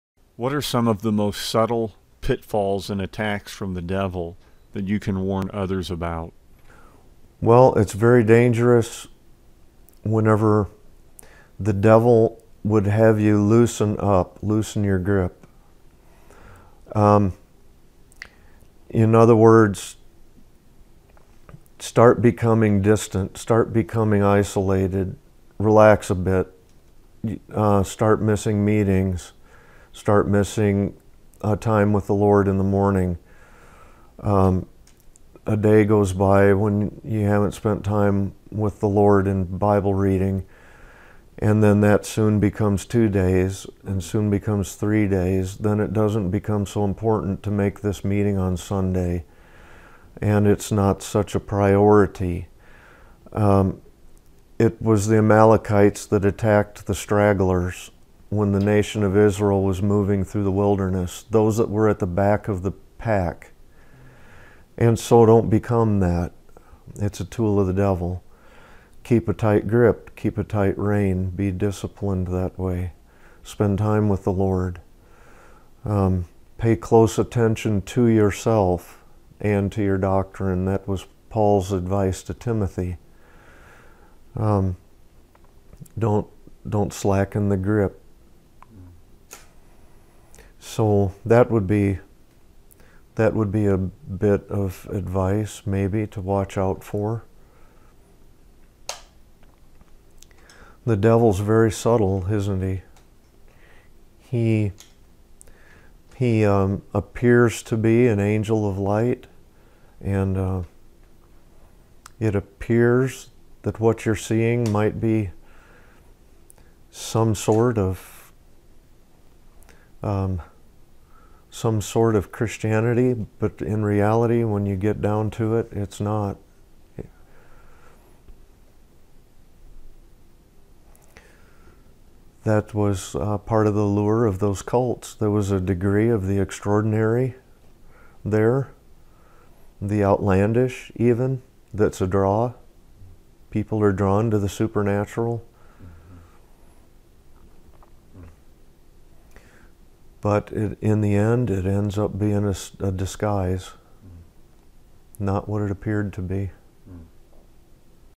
Cristiano: mantente firme en el Señor - Clint Leiter
El enemigo de nuestras almas quiere que nos distanciemos del Señor; que perdamos fuerza cuando se trata de nuestra relación íntima con Cristo y que no hagamos de ella la prioridad principal.

Pregunta: ¿Cuáles son algunas artimañas frecuentes del diablo de las que deben cuidarse los jóvenes cristianos?